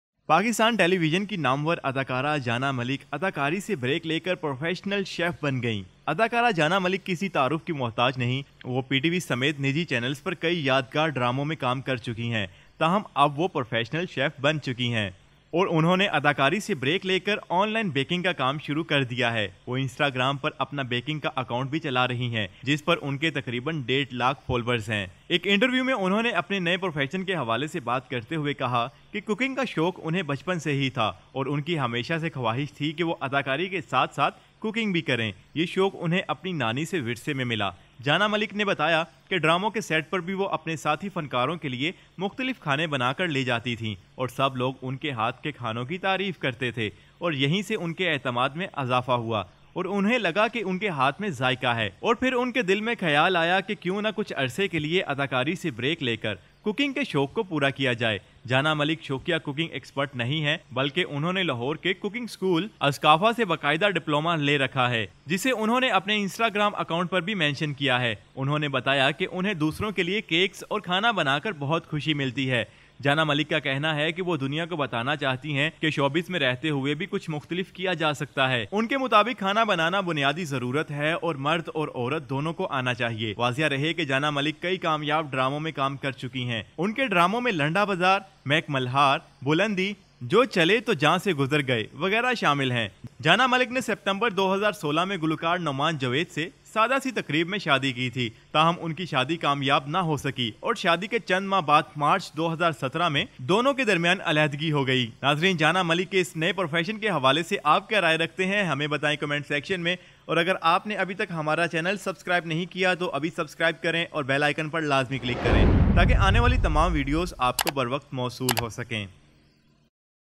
My heart was Filled with Acting | Janan Malik will now be Seen by Fans in Which Sector? | 9 News HD
My heart was Filled with Acting | Janan Malik will now be Seen by Fans in Which Sector?

Karachi (Web Desk) Pakistan Television's famous actress Janan Malik took a break from acting and became a professional chef. Actress Janan Malik does not need any introduction. She has acted in many memorable dramas on private channels including PTV but now she has become a professional chef.

Instagram, which has about 1.5 million followers. In an interview to Urdu News, referring to his new profession, he said that he had been interested in cooking since childhood and he had always wanted to do cooking as well as acting.

Inherited from Janan Malik said that even on the sets of dramas, she used to prepare different dishes for her fellow artists and everyone used to appreciate the food in their hands and from here, their confidence increased and they felt that it was in their hands.

There is taste and then the idea came to his mind why not take a break from acting for a while and fulfill his passion for cooking. Janan Malik is not an amateur cooking expert but has a regular diploma from Coca-Cola School Scaffa in Lahore